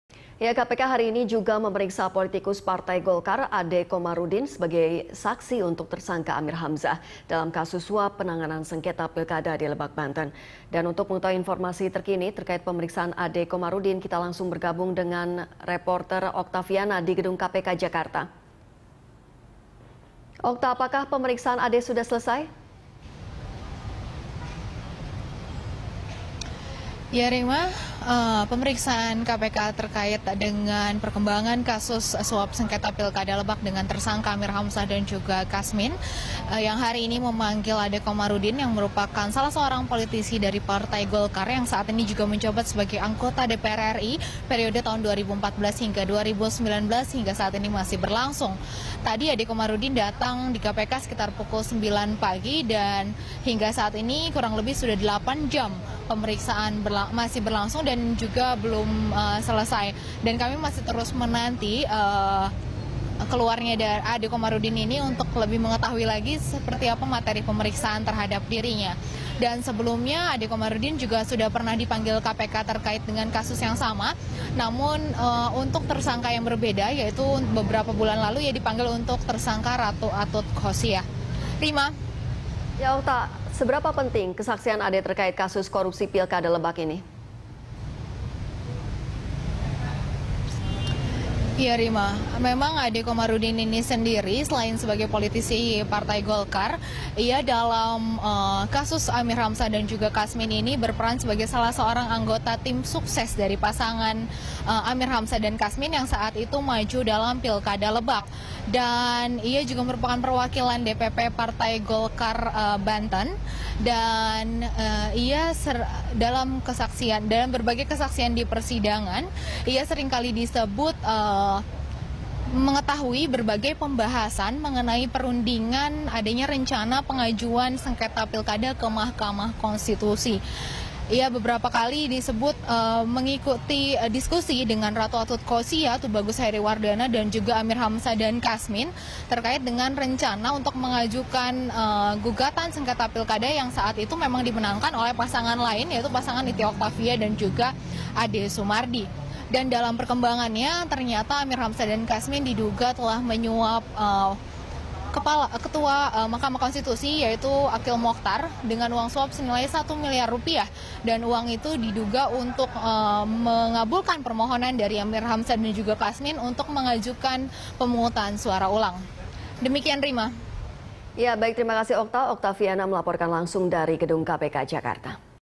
KPK Periksa Ade Komaruddin Terkait Pilkada Lebak
Komisi Pemberantasan Korupsi memeriksa politikus Partai Golkar Ade Komaruddin sebagai sebagai saksi untuk tersangka Amir Hamzah dalam kasus suap penanganan sengketa Pilkada Lebak.

Facebook.com/BeritaSatuTV
Youtube.com/BeritaSatu